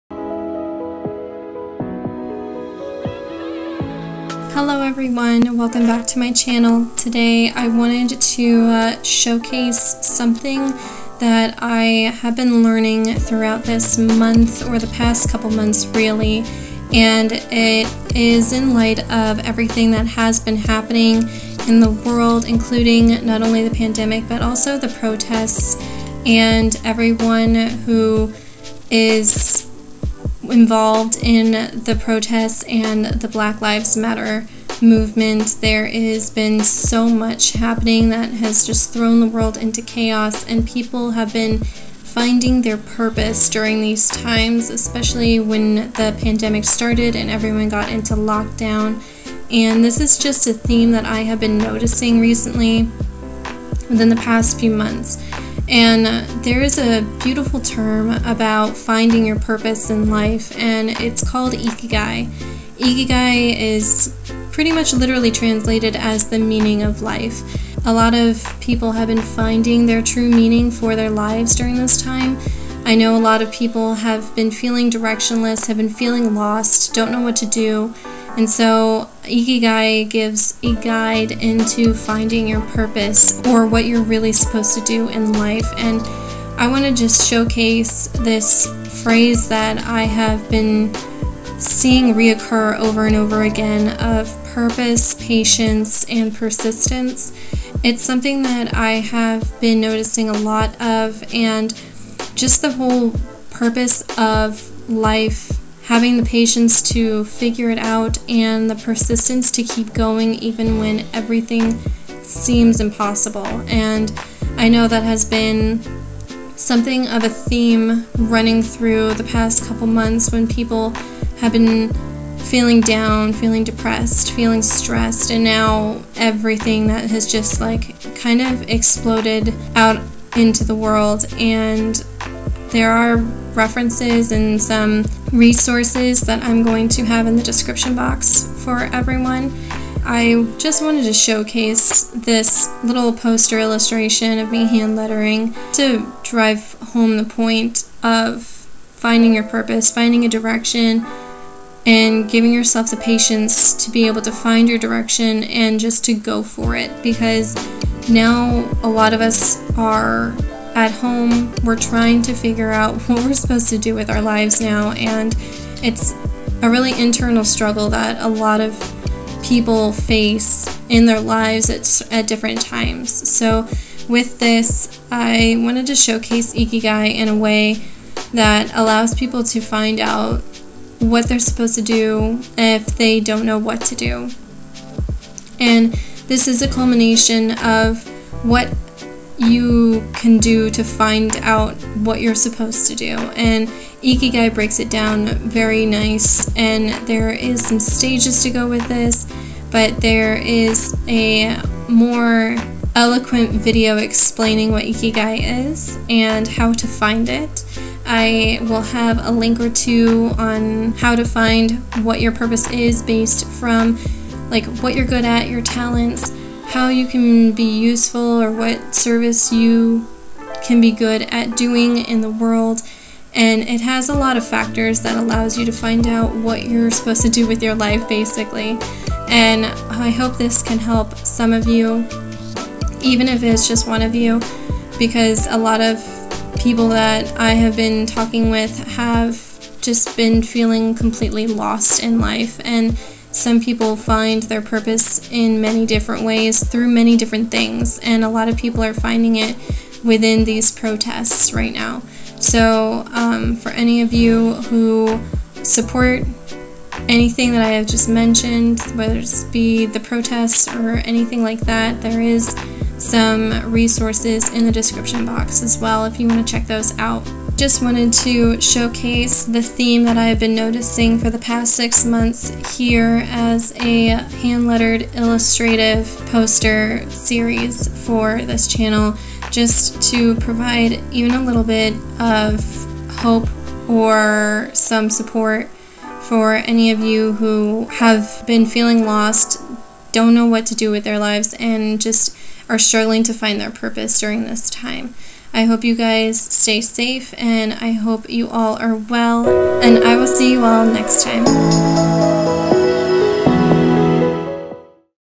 What Is Our Purpose? Weekly Vlog 12
Hello Everyone!

This vlog is a bit different, i wanted to share something I've been learning and thinking about during this time of chaos and uncertainty. That is Purpose, Patience and Persistence. Many have found their purpose within everything happening but others have felt lost, confused and unsure of what they are supposed to do now, hopefully this video will help.
Resources